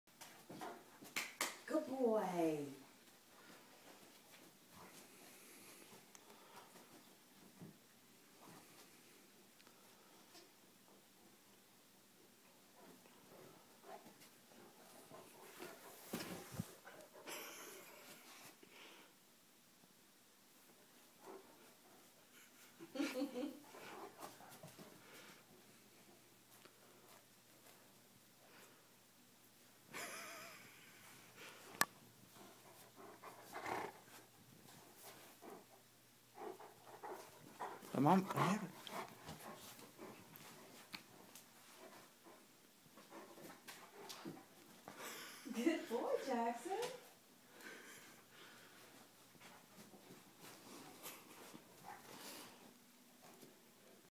Jackson Pyrenees catching his toy
2 1/2 year old Great Pyrenees playing catch with one of his favorite toys.